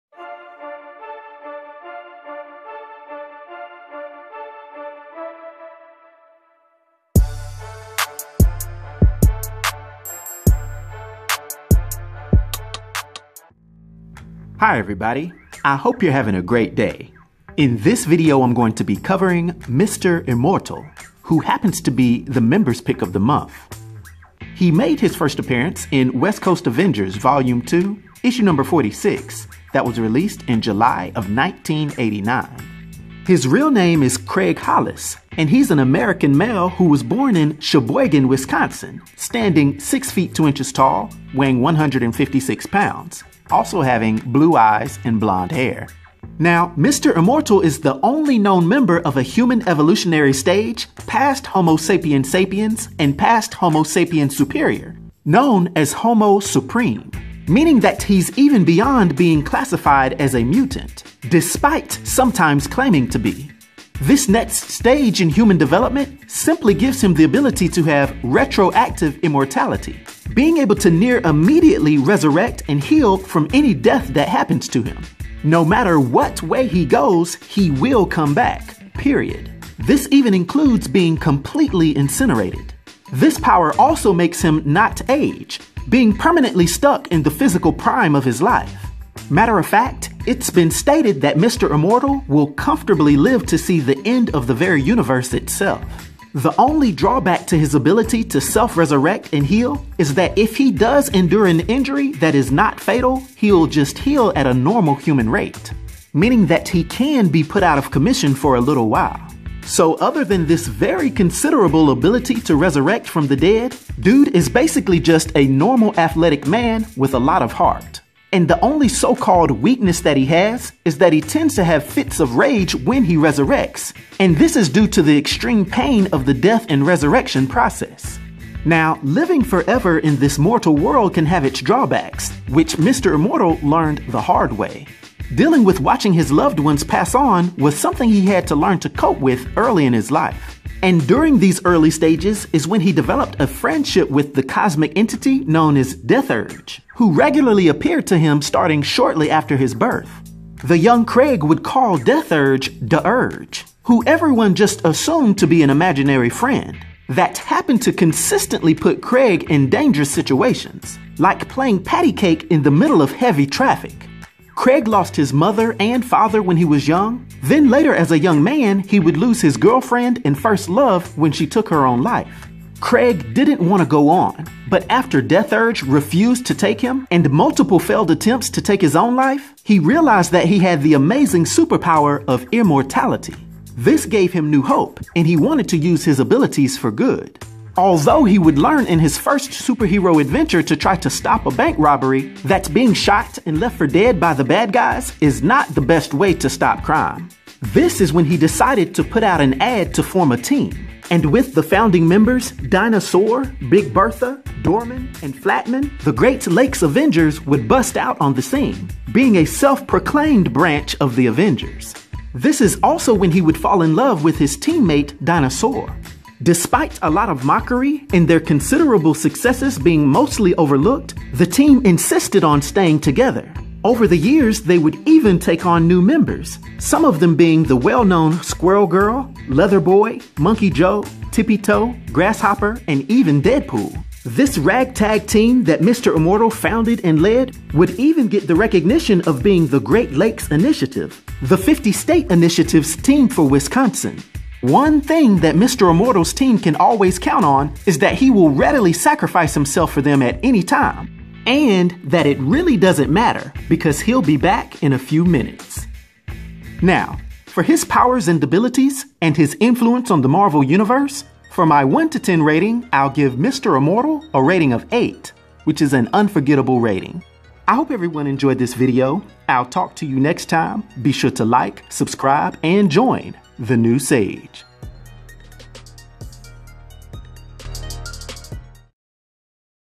Who is Marvel's Mr. Immortal? The Only Known "Homo Supreme" Being.
This video explores the abilities, powers, and origin of the Marvel Comics character "Mr. Immortal." From the young Craig Hollis forming a hazardous bond with the cosmic entity Deathurge, losing those closest to him before learning that he is the only member of the next stage in human development known as Homo Supreme which makes him completely immortal, being able to quickly resurrect from any form of death that he endures, to founding and leading the quirky Great Lakes Avengers team; Let's take a look at this amazing next stage of Marvel superhero, and the most light-hearted true immortal in the Marvel Universe. This includes a short, canon character biography and my own opinionated "Sage Rating," which I use to give ratings of comic book and science fiction characters.